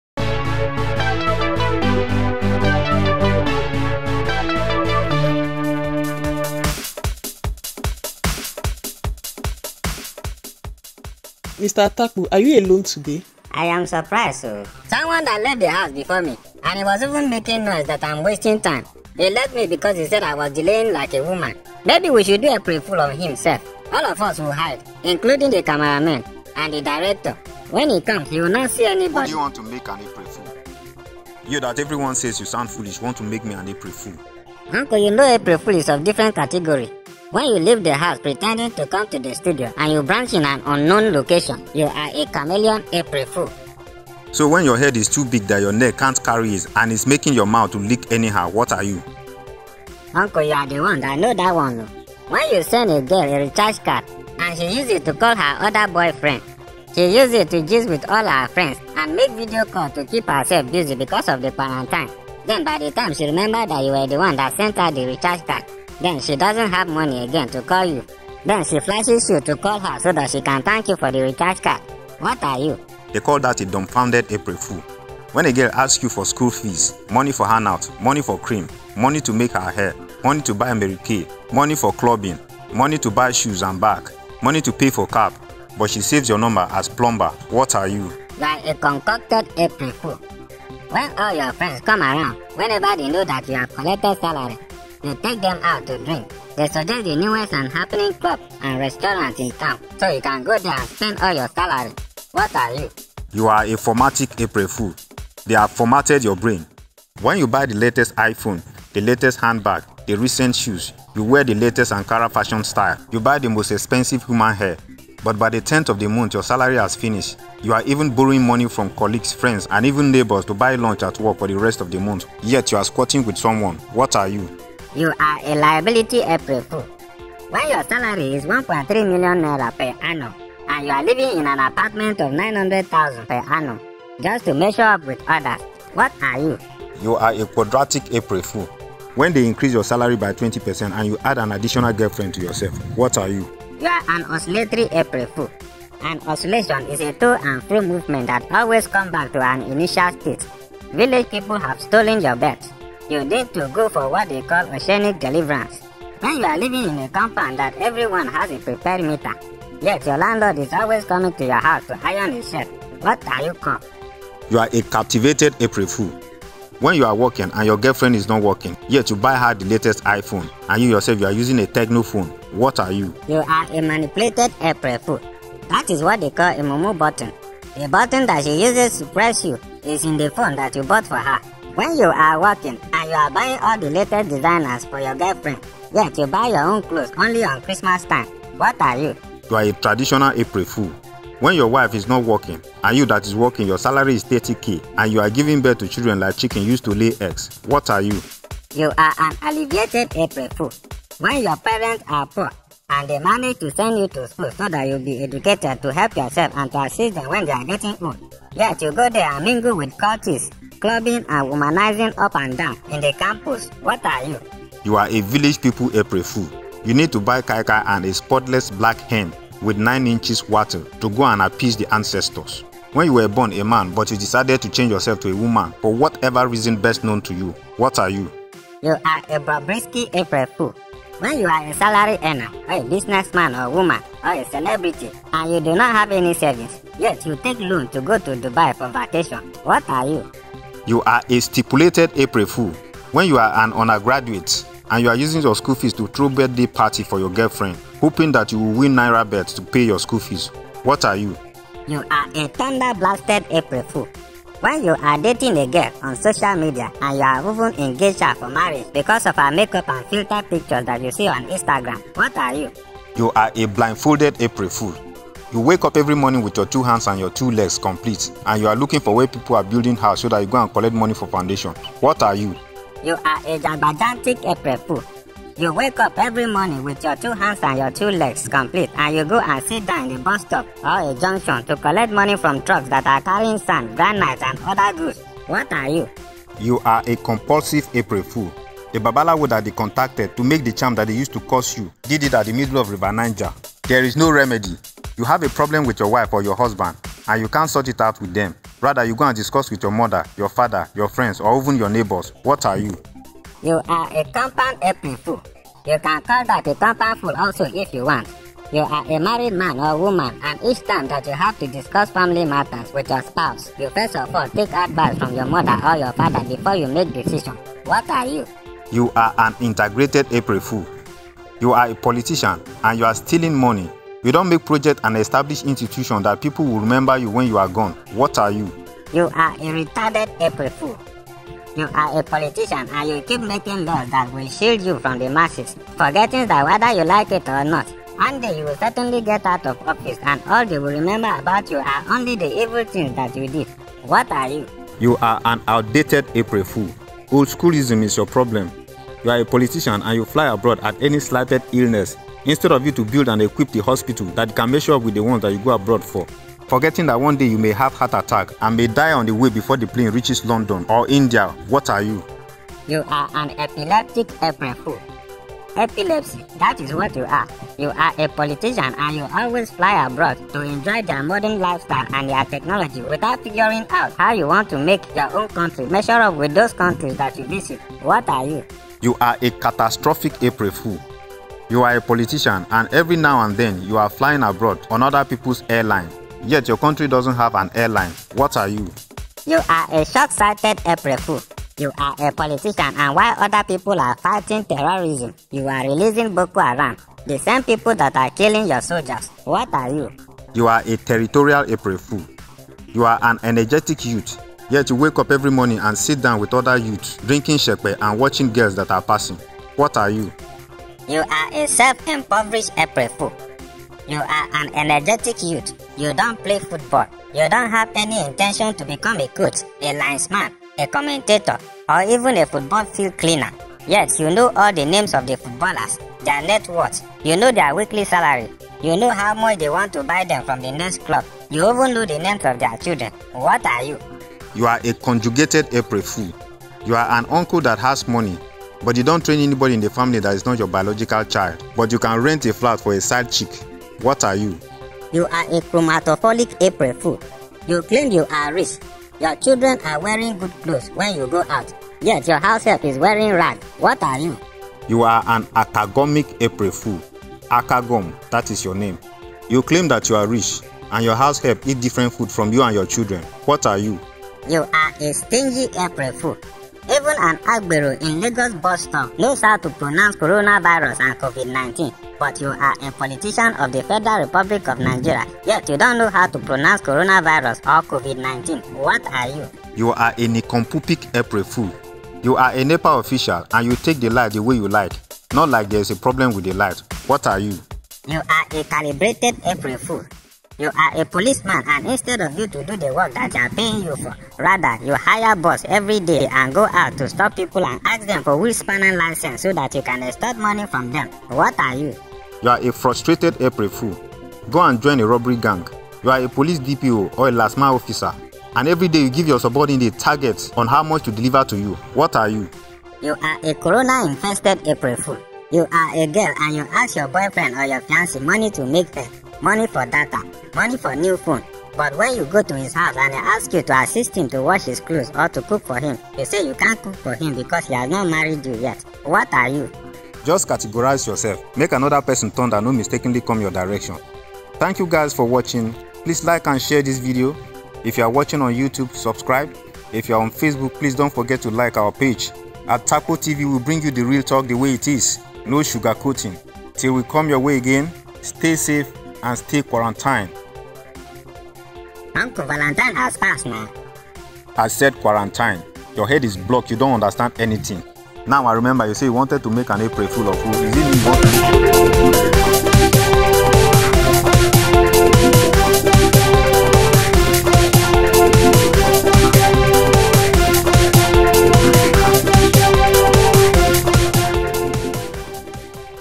April Fool | Takpo TV
April Fool.

What is the real definition of April fool? Watch Takpo and uncle as they breakdown all the categories of April fool